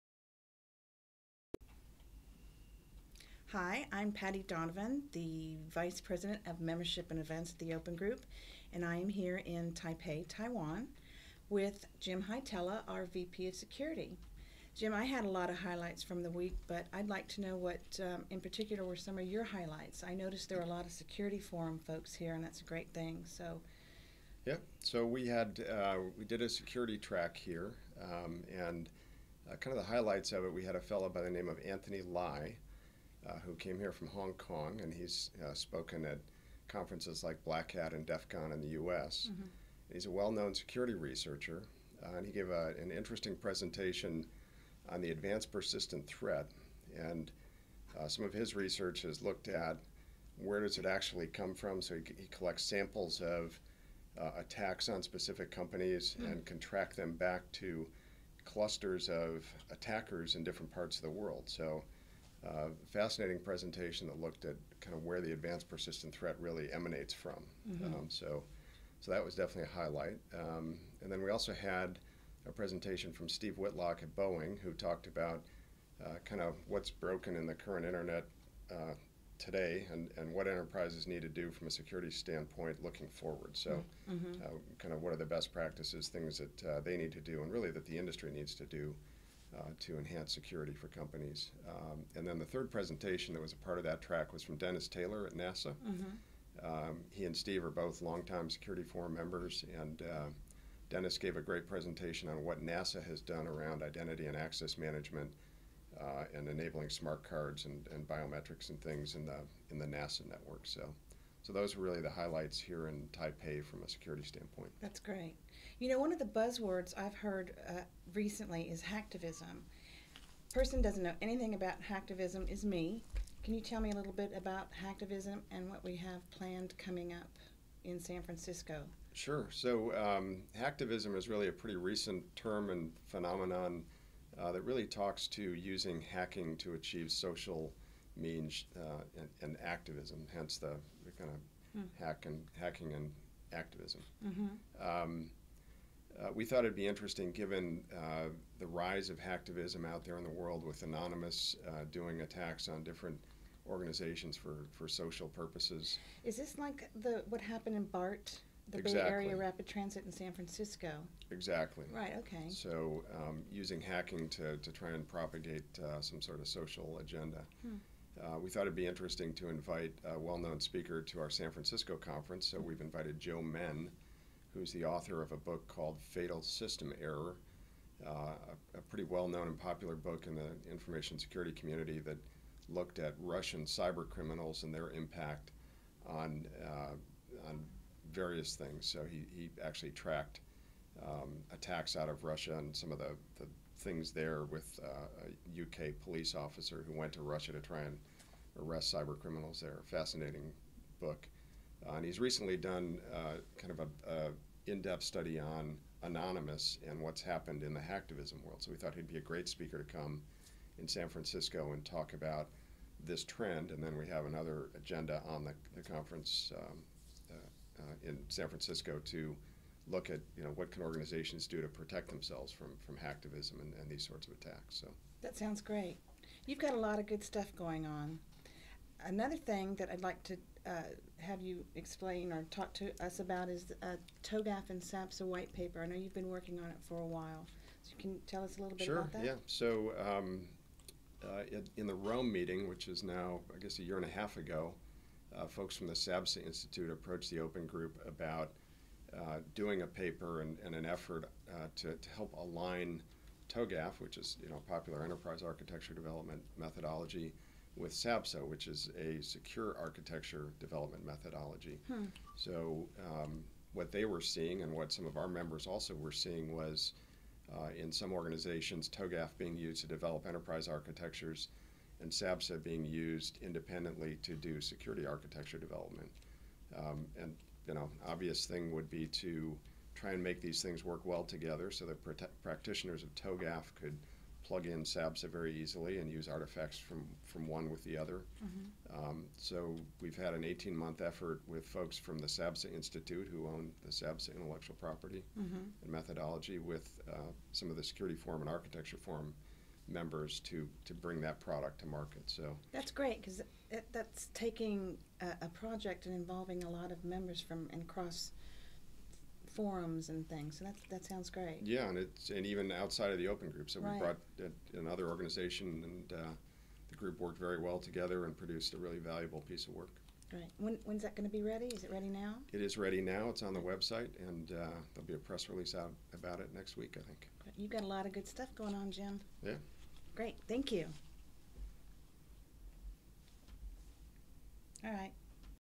Taipei Security Program Highlights
Patty Donovan and Jim Hietala discuss the highlights of the Security program at The Open Group's Taipei conference in October 2011.